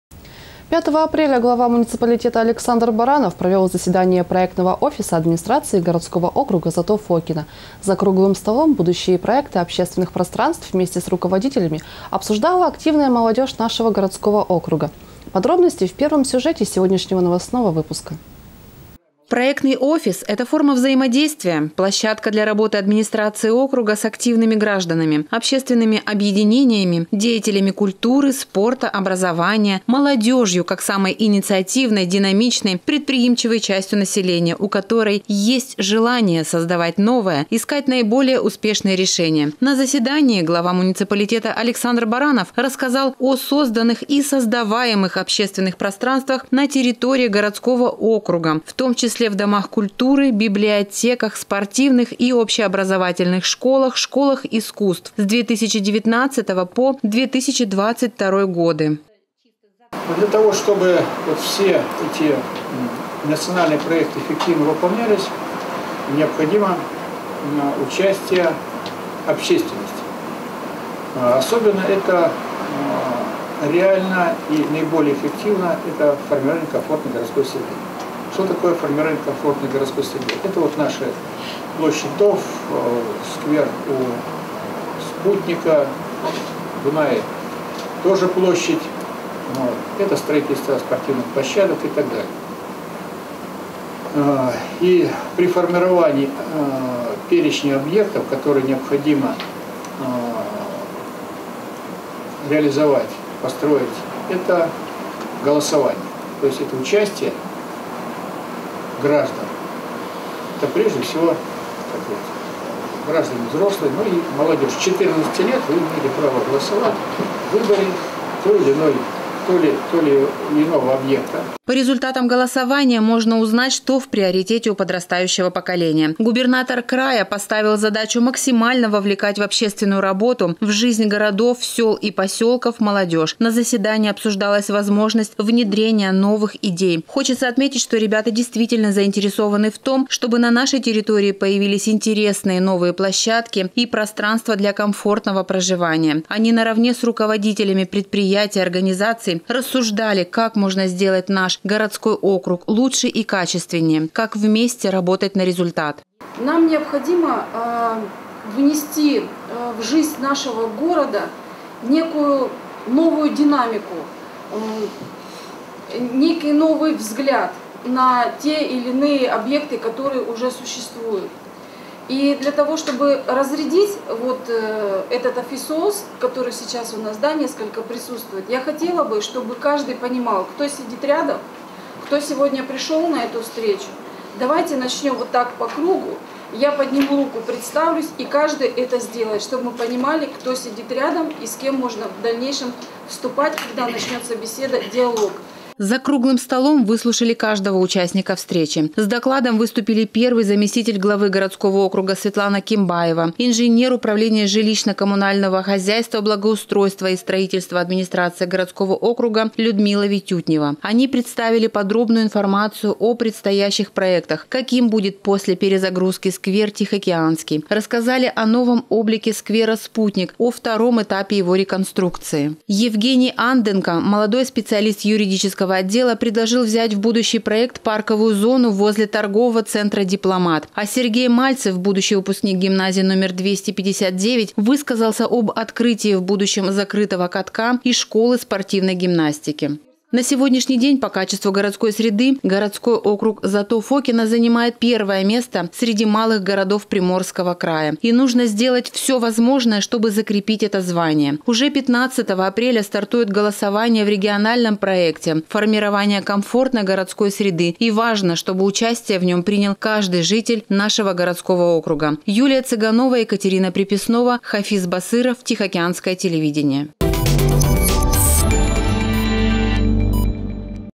РАЗГОВОР НА РАВНЫХ
5 апреля глава муниципалитета Александр Баранов провёл заседание проектного офиса администрации городского округа ЗАТО Фокино. За «круглым столом» будущие проекты общественных пространств вместе с руководителями обсуждала активная молодёжь нашего городского округа.